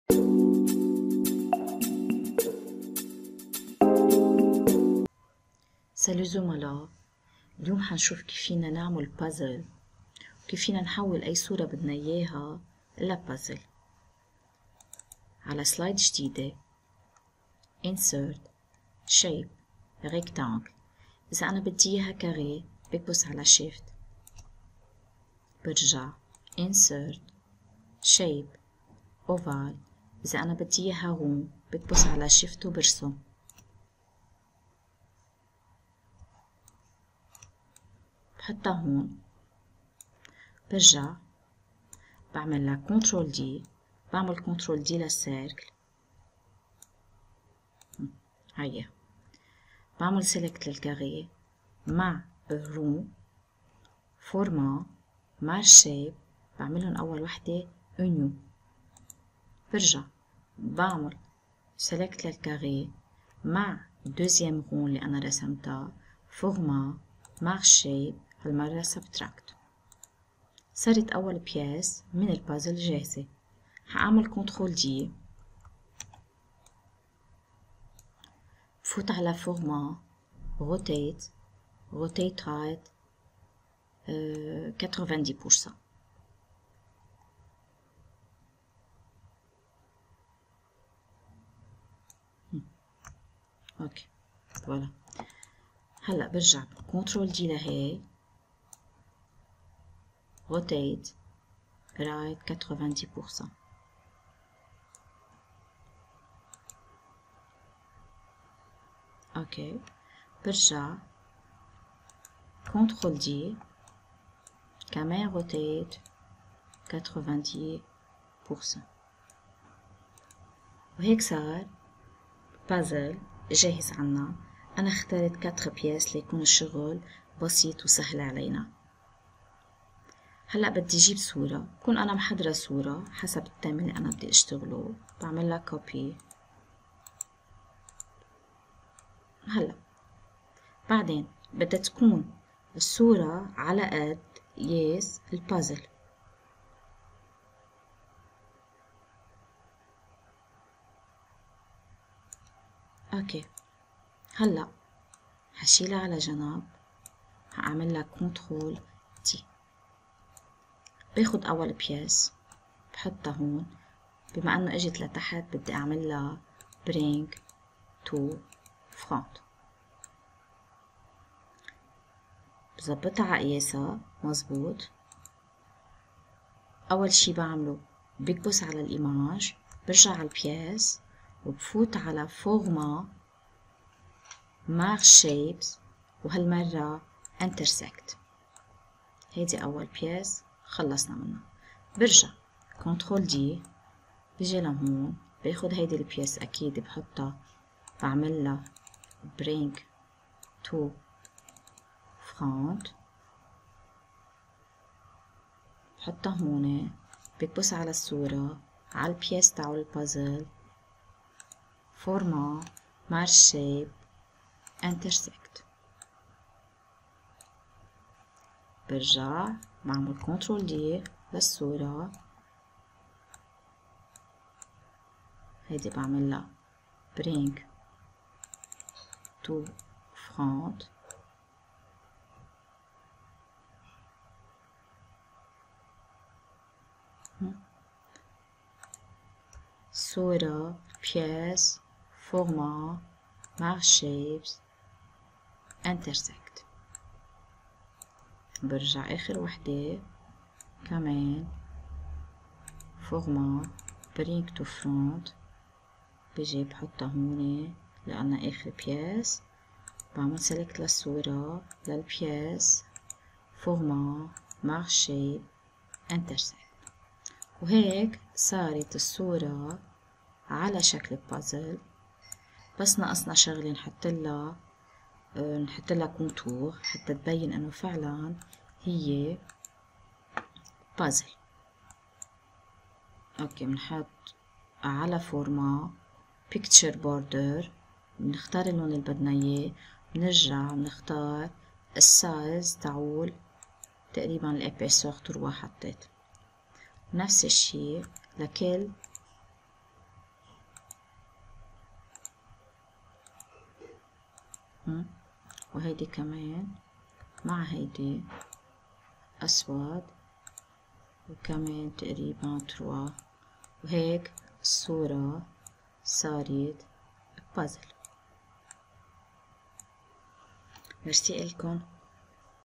How to create Jigsaw Puzzle from Picture in PowerPoint
If you wanted to create a puzzle piece image from an existing image, you would think this was something that could only be done in Photoshop or pay someone to do it. With most versions of PowerPoint this can be done in just a few steps.